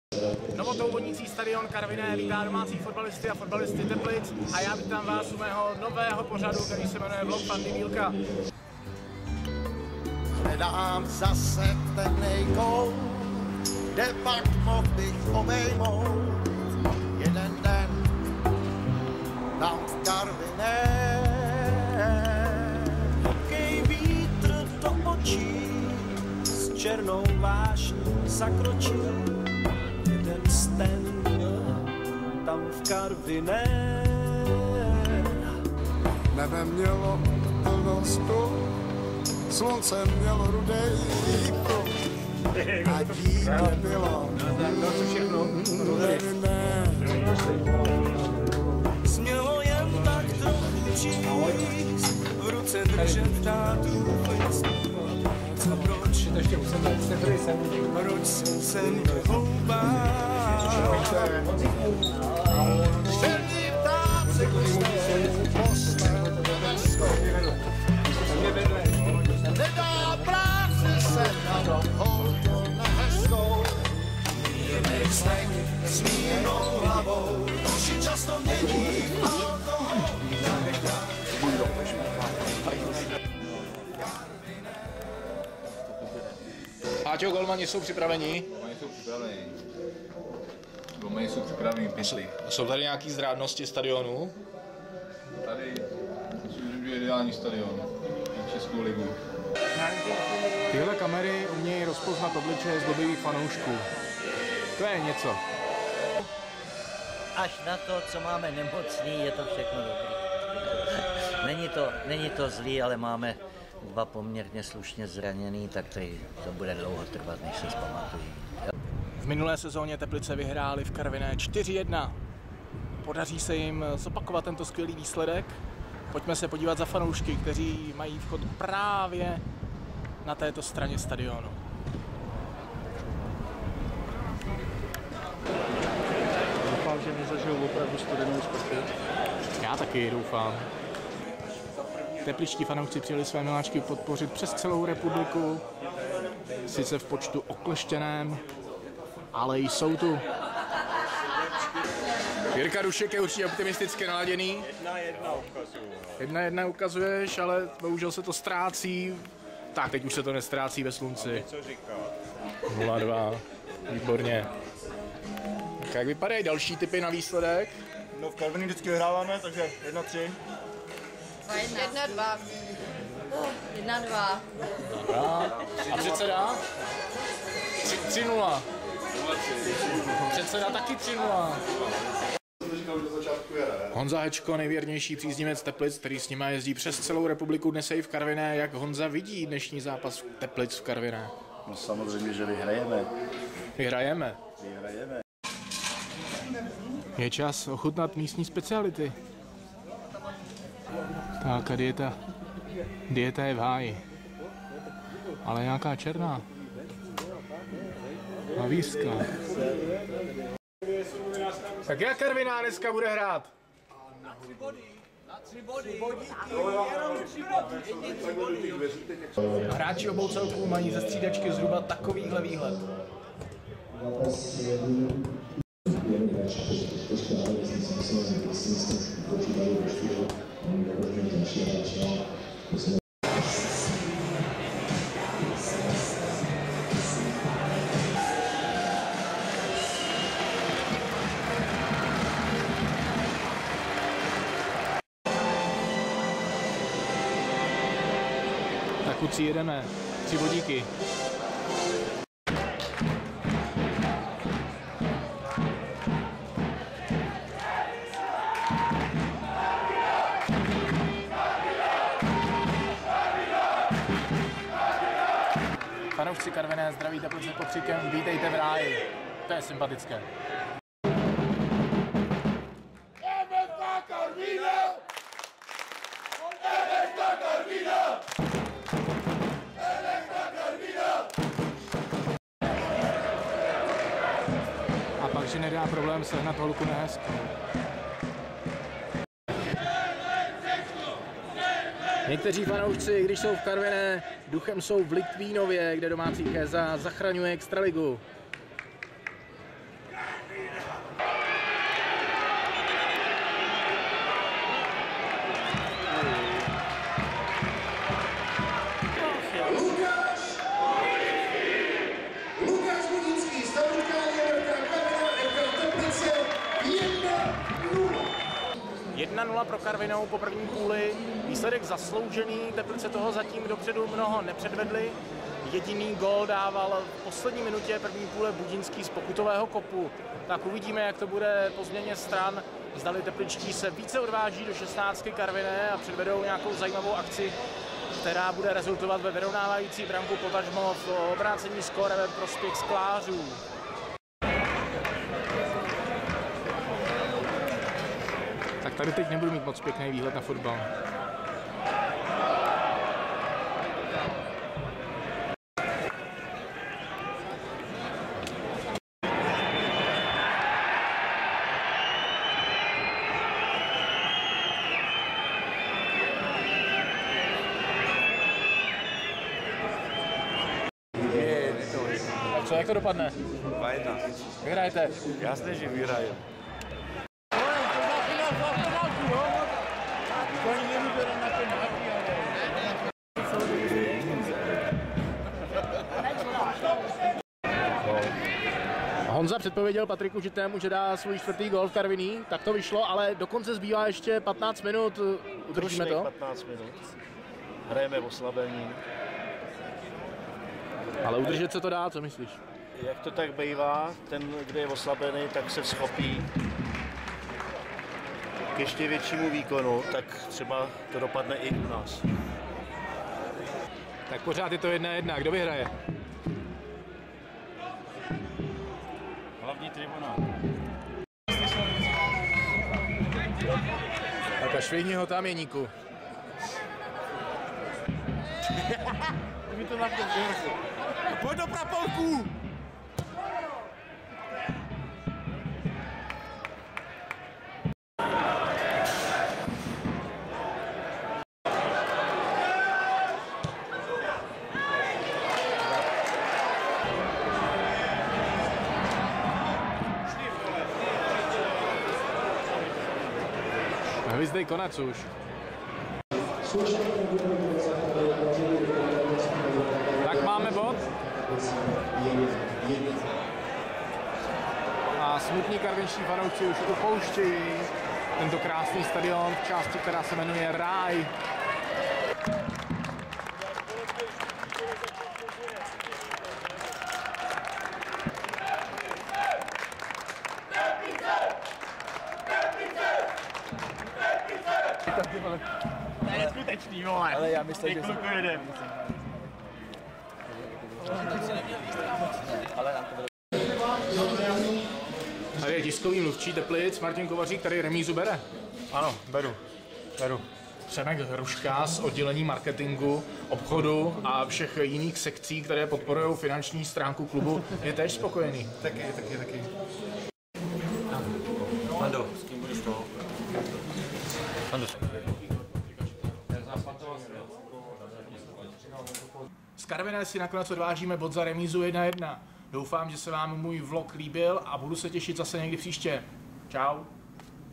Vlog Fandy Bílka - Tam v Karviné
Zápas 25. kola HET ligy Karviná - Teplice (1:1) kamerou Fandy Bílka.